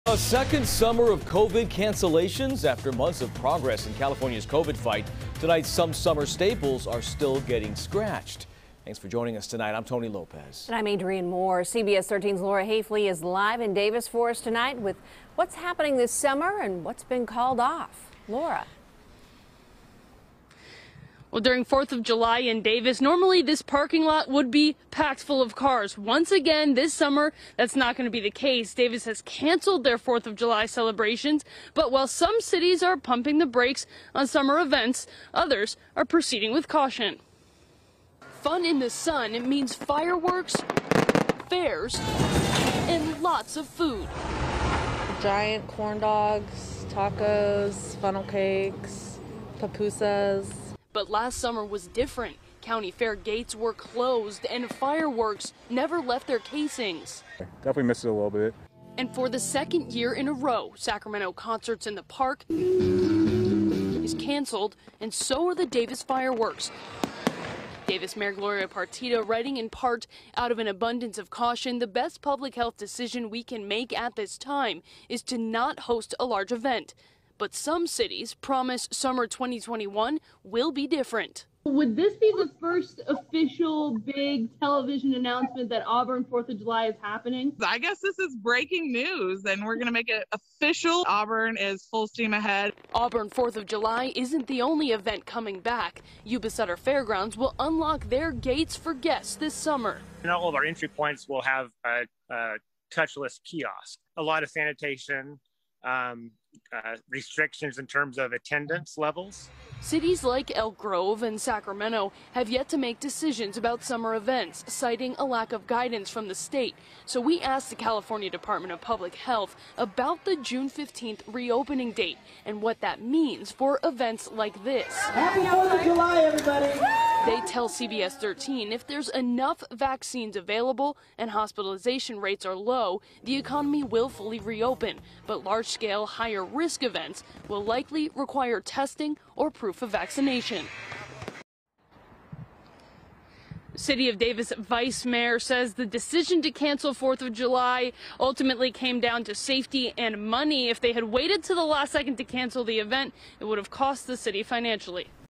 Local Cities Divided On Summer Events As Restrictions Ease Up
Summer is right around the corner and cities across the Sacramento region are making decisions about which events will take place and which events won’t as loosened restrictions allow for more activities.